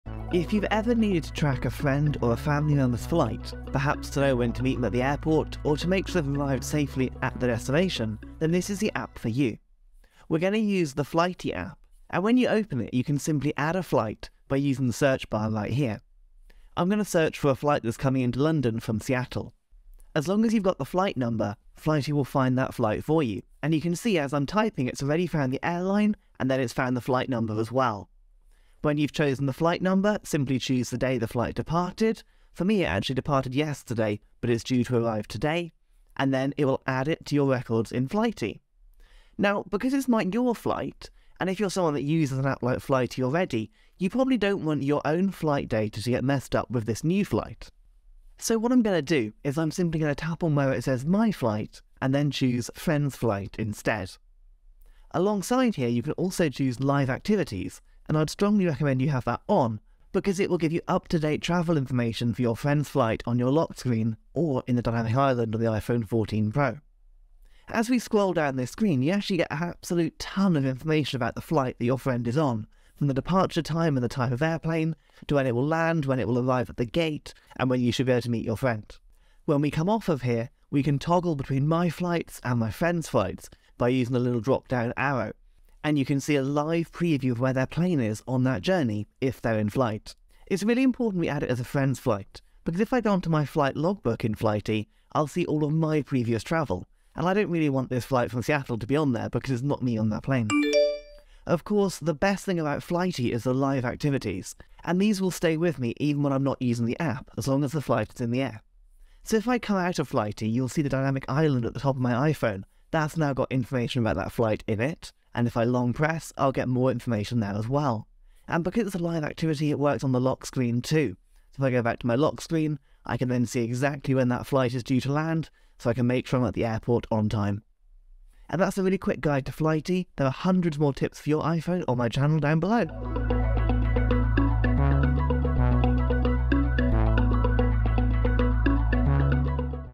How to Track Your Loved One's Flight in Real-Time with Flighty App
Are you tired of constantly refreshing the airline's website to track your loved one's flight? Look no further than the Flighty app! In this tutorial video, I'll show you how to use the Flighty app for iPhone to track your friend or family member's flight in real-time. From departure and arrival times to gate changes and delays, Flighty keeps you up-to-date every step of the way. Say goodbye to the stress of travel and hello to peace of mind with Flighty. Join me as I walk you through step-by-step and show you just how easy it is to track your loved one's flight with Flighty. Don't miss out on this ultimate guide to flight tracking!